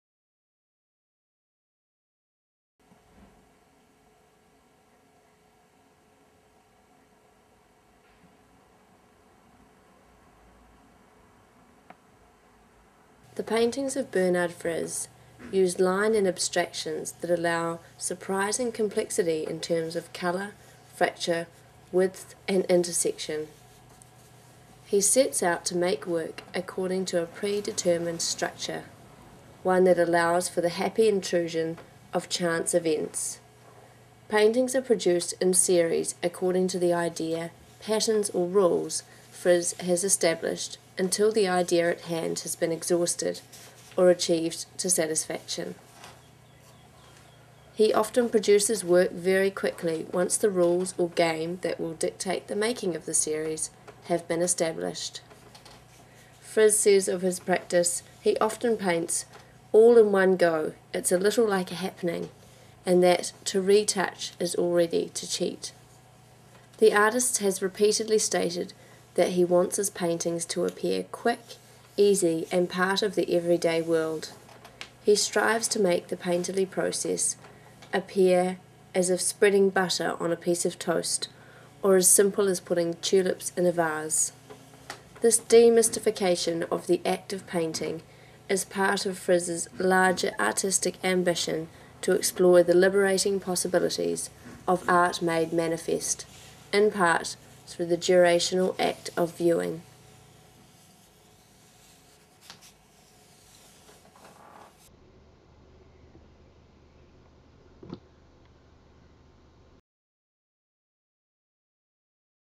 Bernard Frize's mold removal technique with Melissa Durbin
This video is part 6 in the Artists help with the housework series by Melissa Durbin. The series takes artists techniques and theories and applies them to mundane household chores. Using a variety of artists I hope to connect art theory with the everyday and comment on the challenges of art making in a contemporary context.